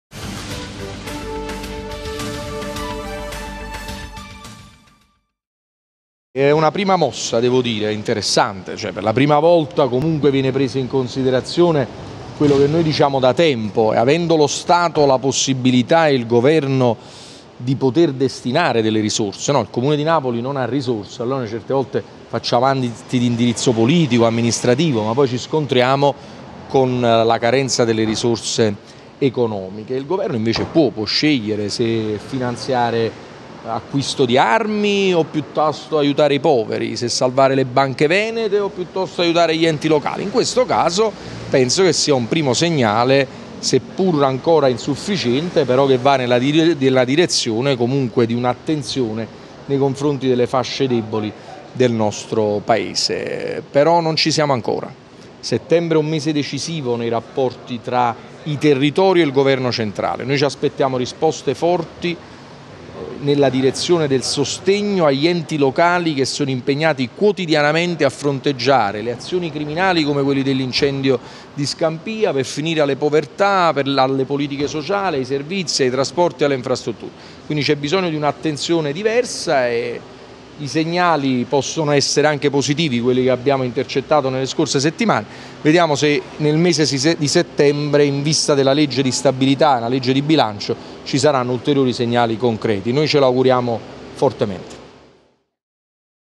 De Magistris reddito d’inclusione, un primo segnale interessante
Il sindaco di Napoli Luigi de Magistris commenta il reddito d’inclusione introdotto con un decreto legislativo approvato dal governo.